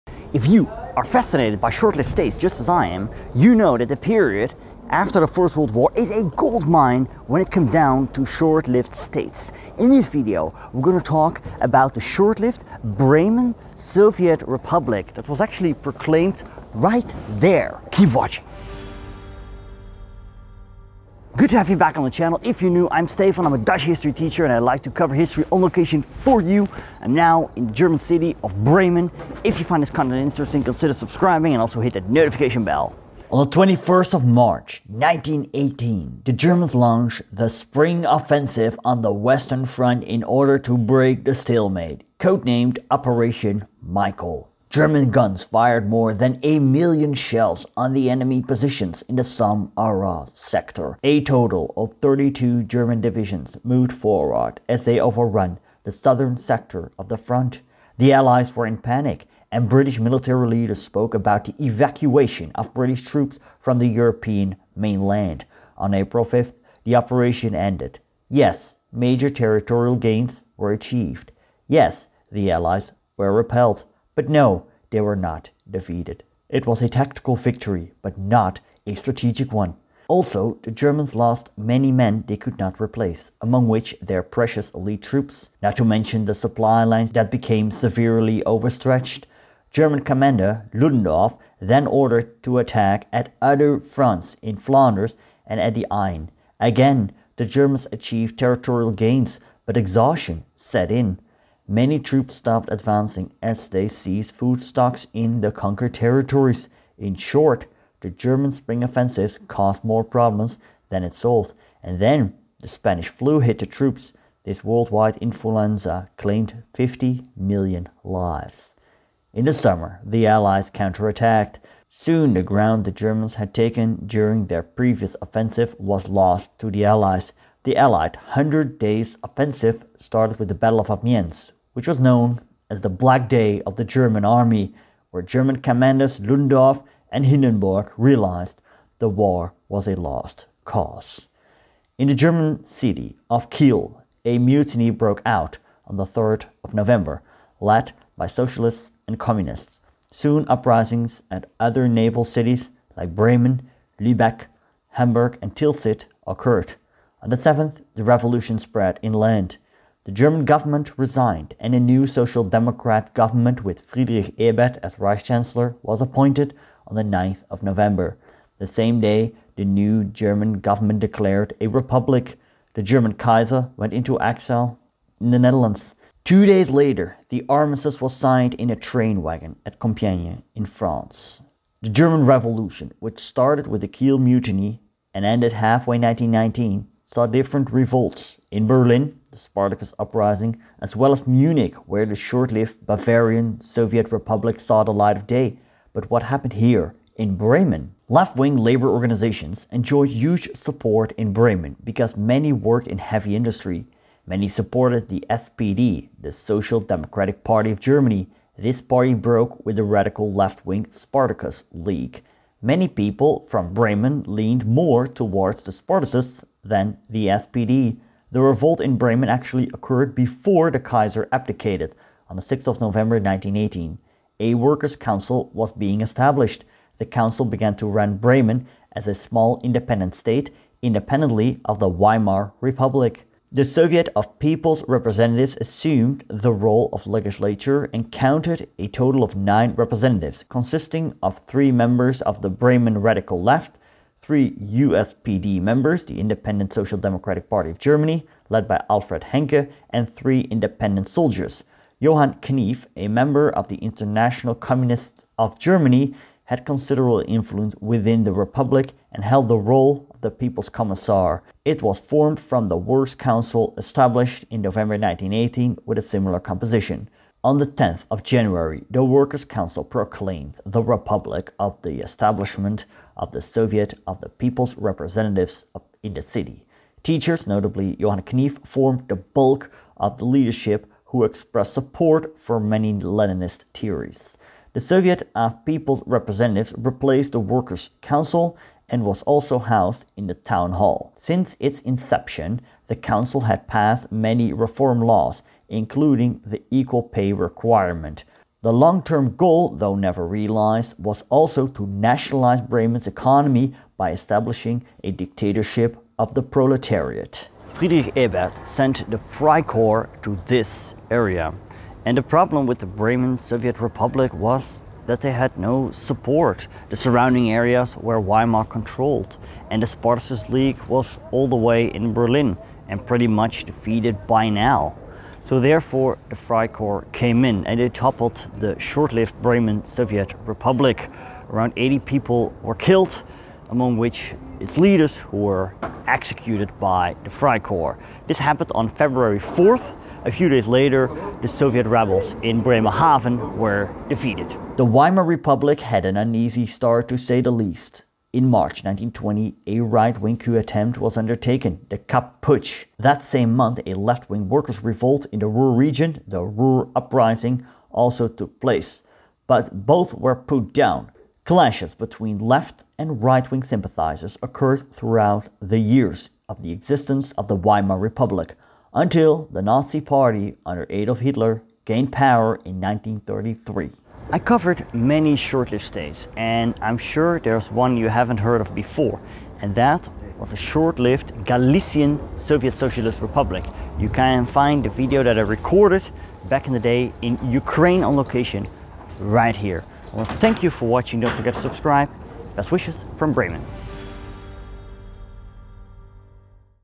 The Bremen Soviet Republic (1919) – A Short-Lived Communist State during the German Revolution
During the period and aftermath of the German Revolution (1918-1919) different short-lived states saw the light of day. Well known is the Bavarian Soviet Republic. Lesser known is the Bremen Soviet Republic (Bremer Räterepublik) that was proclaimed early 1919. It was a short-lived socialist state that challenged the Weimar Republic. Eventually this state was toppled by the Freikorps.
History Hustle presents: The Bremen Soviet Republic (1919) – A Short-Lived Communist State during the German Revolution.

SOURCES
– The Vanquished. Why the First World War Failed to End, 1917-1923 (Robert Gerwarth).
– The First World War (John Keegan).

IMAGES
Images from commons.wikimedia.org.

SOUNDS
Freesound.org.